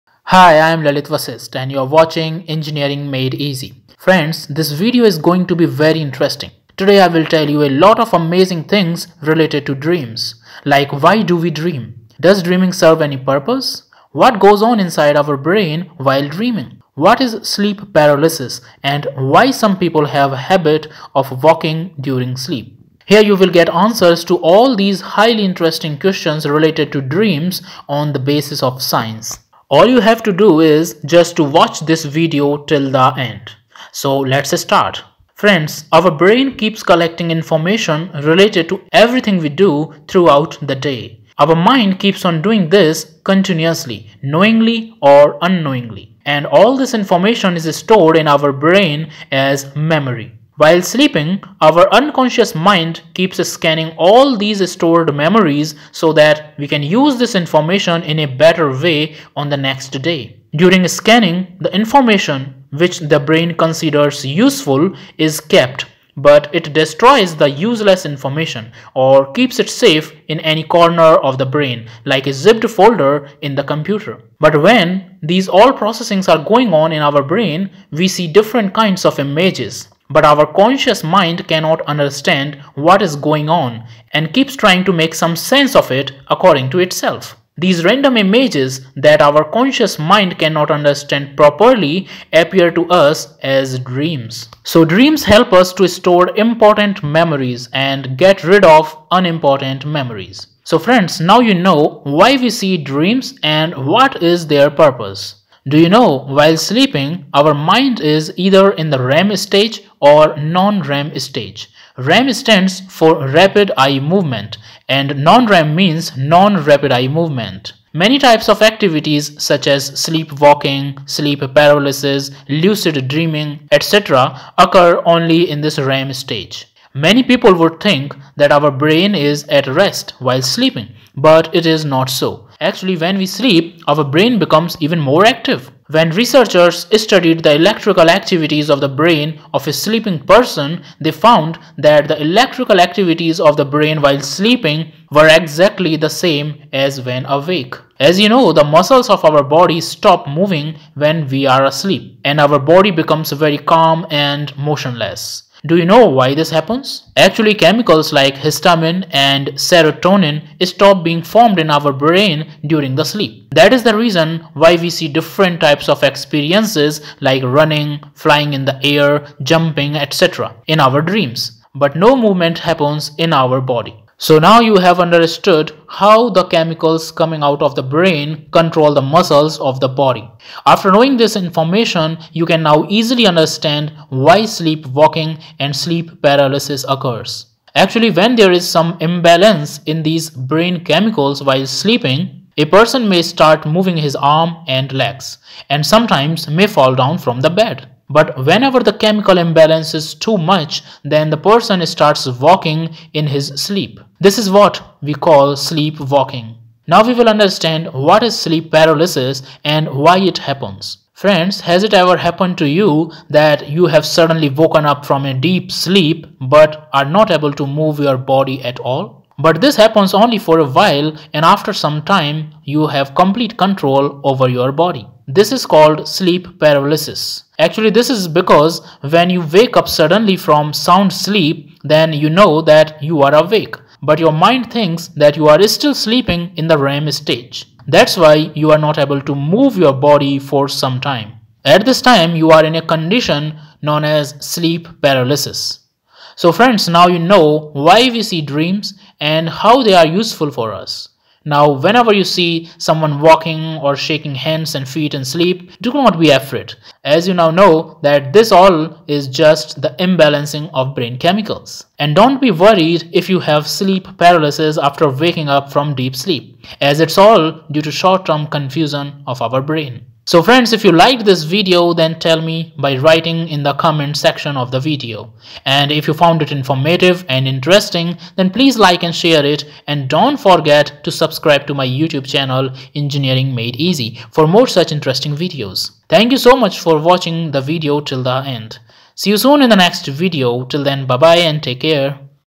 Why Do We Dream - What is Sleep Paralysis
This Video is about amazing Facts about dreams. Like, why do we dream? What is the purpose of dreams? What goes on inside our brain while dreaming. What is sleep paralysis, Lucid Dreams and why some people have habit of walking during sleep (Night Walking). We will also see here, What researches and Studies are being conducted on dreams. you will get answers to all of these highly interesting questions related to dreams on the basis of Science.
Friends, Our brain keeps collecting information related to everything we do throughout the day. Our mind keeps on doing this continuously, knowingly or unknowingly. And all this information is stored in our brain as memory.
While sleeping, our unconscious mind keeps scanning all these stored memories so that we can use this information in a better way on the next day. During scanning, the information which the brain considers useful, is kept but it destroys the useless information or keeps it safe in any corner of the brain like a zipped folder in the computer.
But when these all processing are going on in our brain, we see different kinds of images, but our Conscious Mind cannot understand what is going on and keeps trying to make some sense of it according to itself. These random images that our conscious mind can not understand properly appear to us as dreams.
So dreams help us to store important memories and get rid of unimportant memories.
So Friends now you know why we see dreams and what is their purpose.
Do you know, At bedtime, our mind is either in the REM stage or NON REM Stage. REM stands for Rapid Eye Movement and NON REM means Non Rapid Eye Movement.
Many types of activities such as sleep walking, sleep paralysis, lucid dreaming etc. occur only in this REM stage.
Many people would think that our mind is at rest while sleeping, but it is not so.  Actually, when we sleep, our brain becomes even more active!!!
 When researchers studied the electrical activities of the brain of a sleeping person, they found that the electrical activities of the brain while sleeping were exactly the same as when awake.
As you know, the muscles of our body stop moving when we are asleep and our body becomes very calm and motionless. Do you know why this happens?
Actually, chemicals like histamine and serotonin stop being formed in our brain during the sleep. That is the reason, why we see different types of experiences like running, flying in the air, jumping etc. in our dreams, but no Movement happens in our body.

So now you have understood how the chemicals coming out of the brain control the muscles of the body.
After knowing this information, you can now easily understand why sleep walking and sleep paralysis occurs.
Actually, when there is some imbalance in these brain chemicals while sleeping, a person may start moving his arms and legs and sometimes may fall down from the bed. But whenever the chemical imbalance  is too much, then the person starts walking in his sleep. This is what we call sleep walking.
Now we will understand what is sleep paralysis and why it happens?

Friends, has it ever happened to you that you have suddenly woken up from a deep sleep but are not able to move your body at all ?
But this happens only for a while and after sometime you have complete control over your body.  Friends, this is called sleep paralysis. Actually, this is because, when you wake up suddenly from sound sleep, then you know that you are awake but your mind thinks that you are still sleeping in the REM stage. That's why you are unable to move your body for some time. At this time you are in a condition known as Sleep Paralysis.
So friends now you know why we see dreams, and how they are useful for us.
Now whenever you see someone walking or shaking hands and feet in sleep, do not be afraid, as you now know that this all is just the imbalancing of brain chemicals. And don't be worried if you have sleep paralysis after waking up from deep sleep, as it's all due to short- term confusion of our brain.